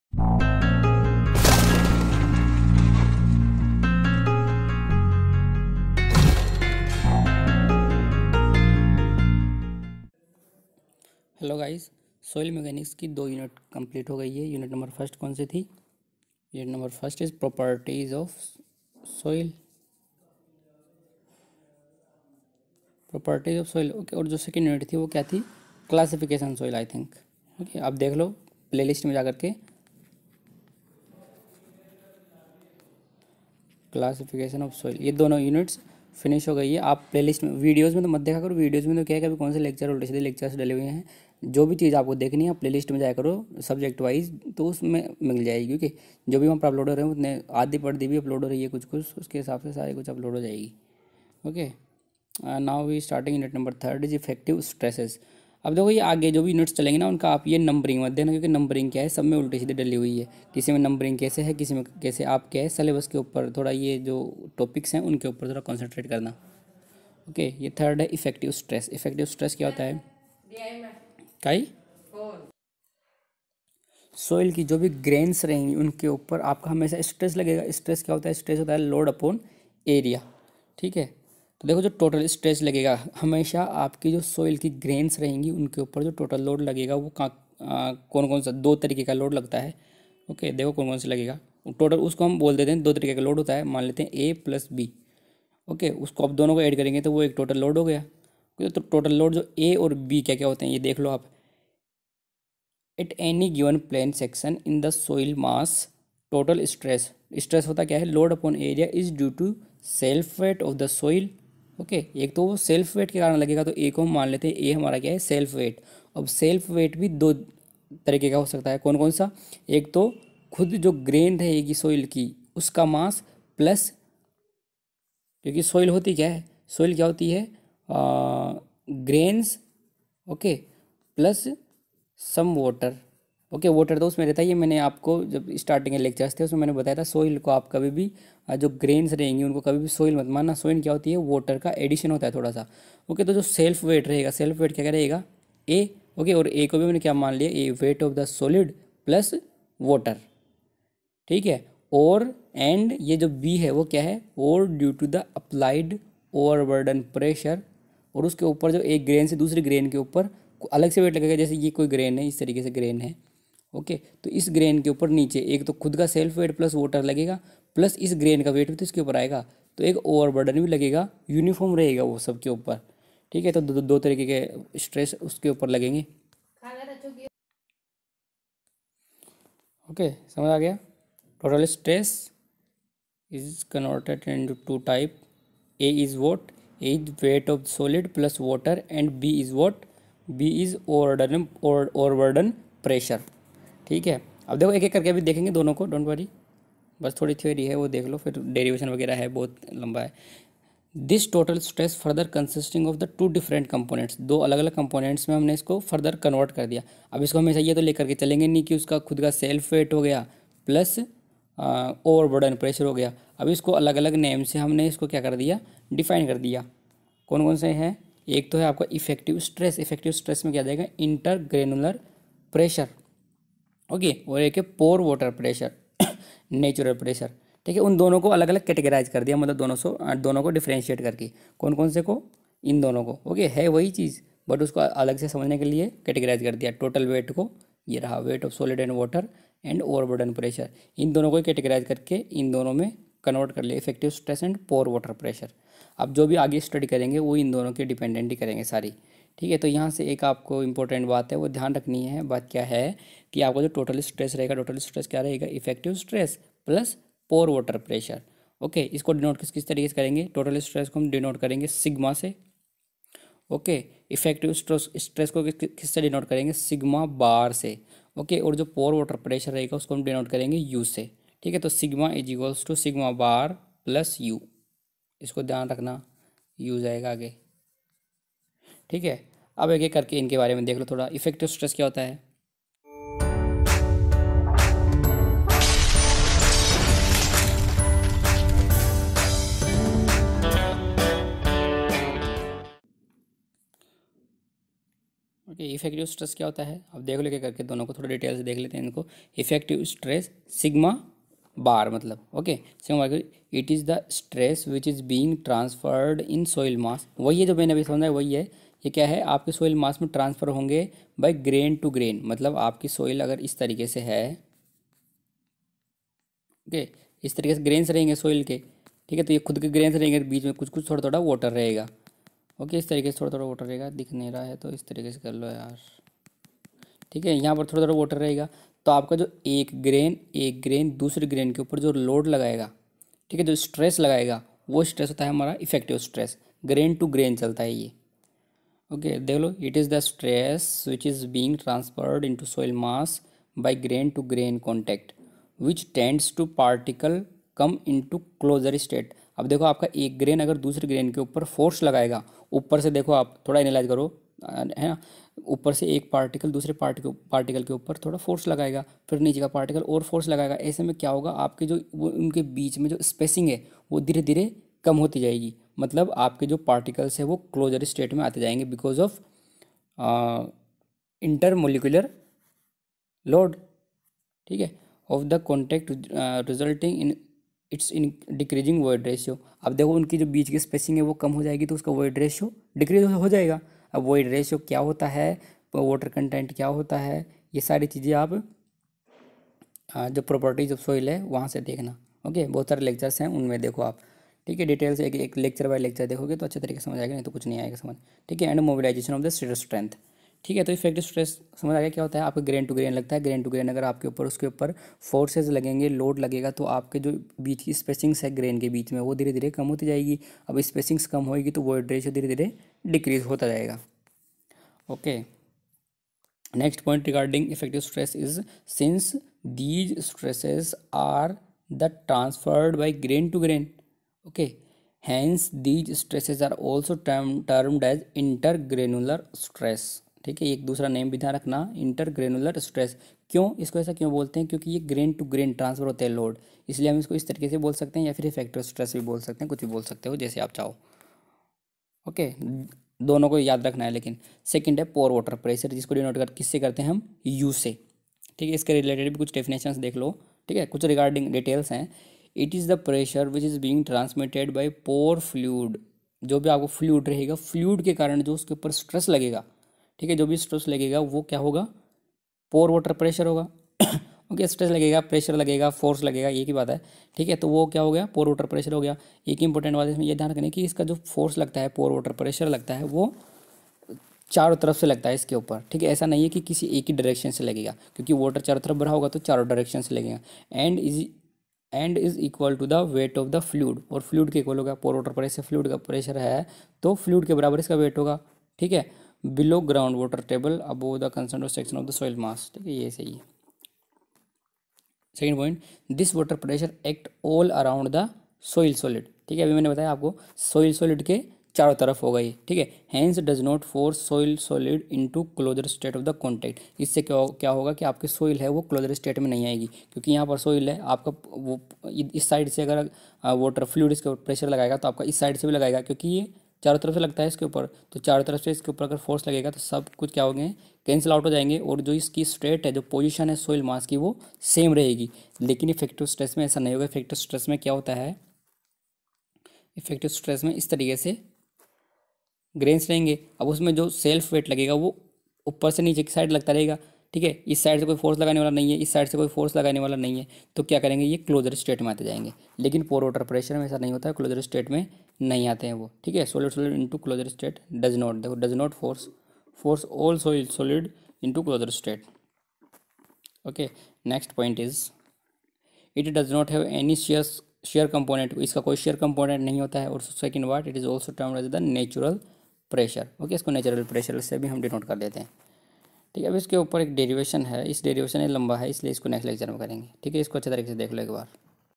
Soil Mechanics~Lec 17~U3~Effective Stresses (Pore Water Pressure) by Bharat Kumar Mahawar
For Purchase PDF Files and Many More in Low Amount